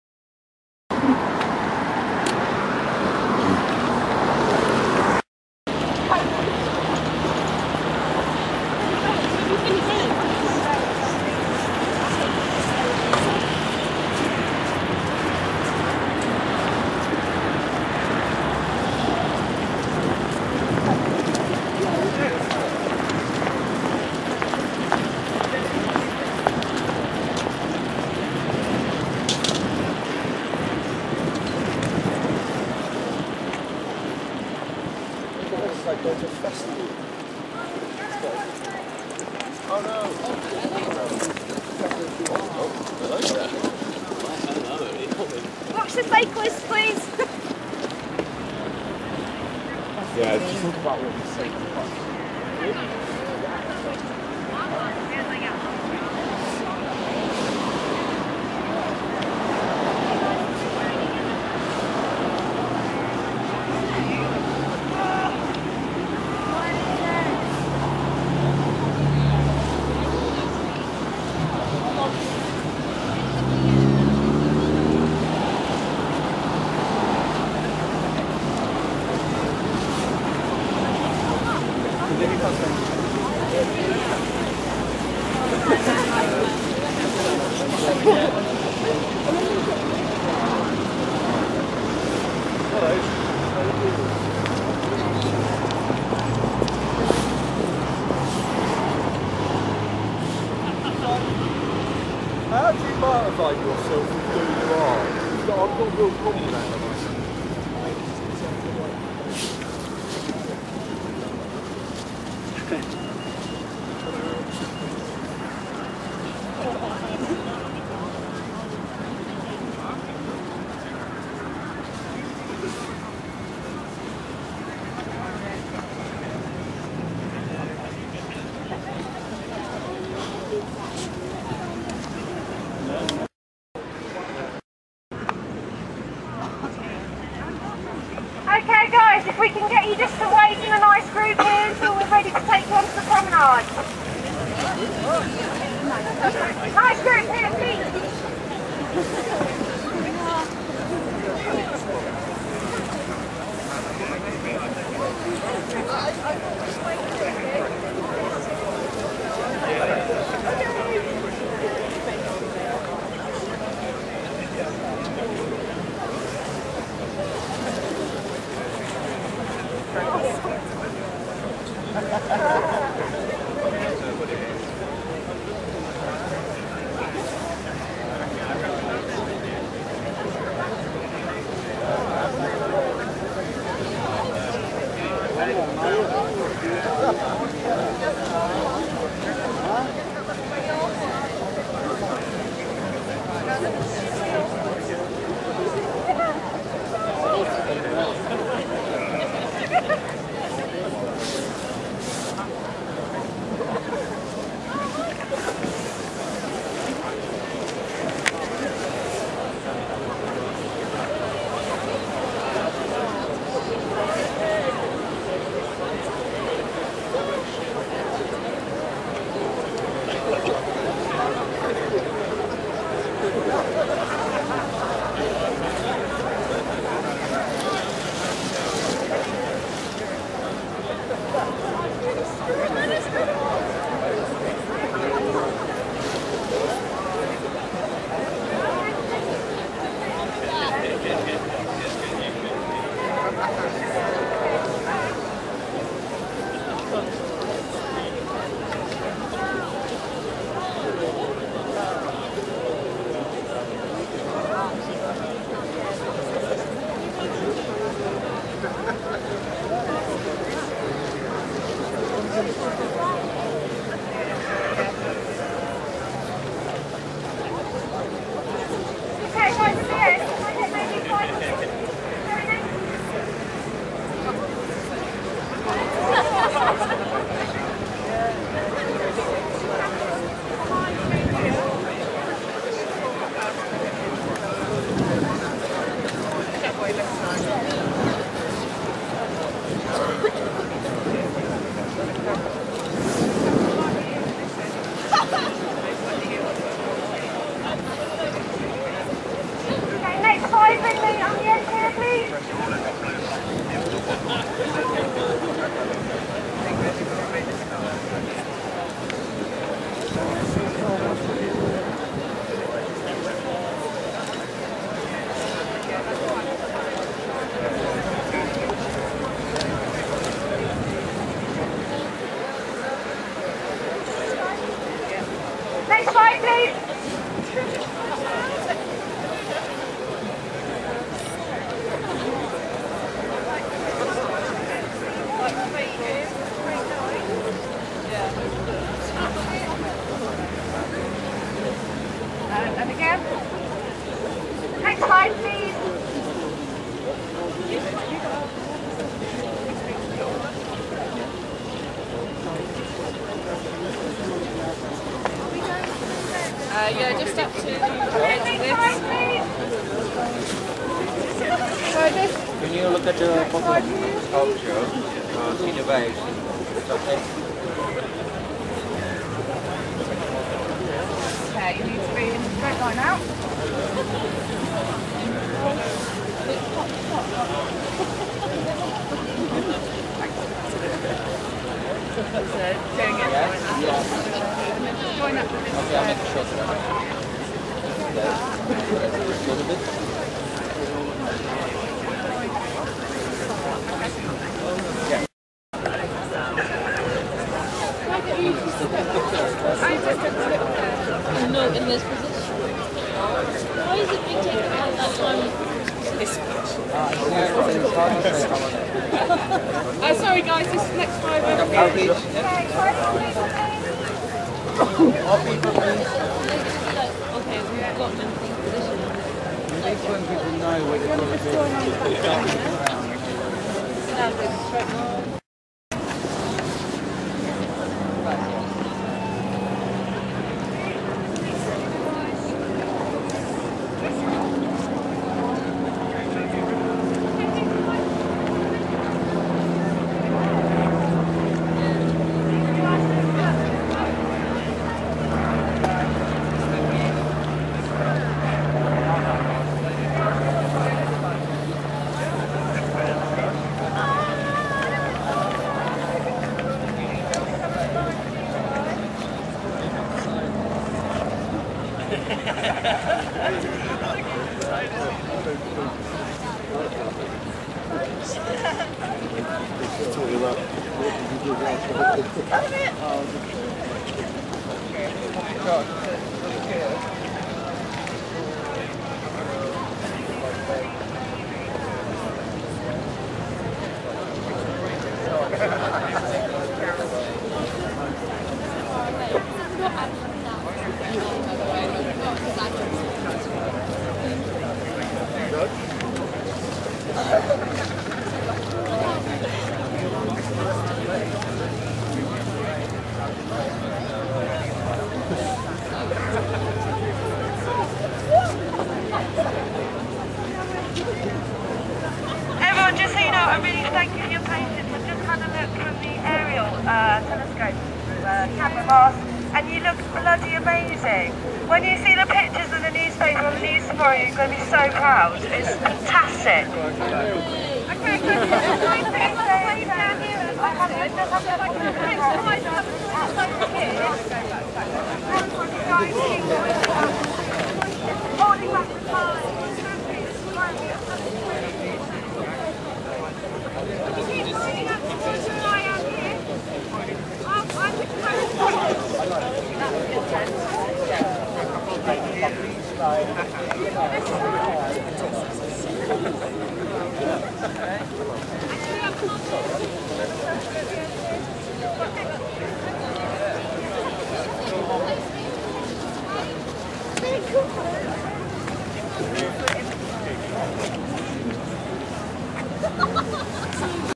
Making of Cnut the Great 1/4
A human sculpture recreation of the legendary King Canute trying to hold back the tides,but failing to command the seas, created by Thom Yorke and Stanley Donwood on the 350 EARTH Art project Hove Lawns /promenade Brighton.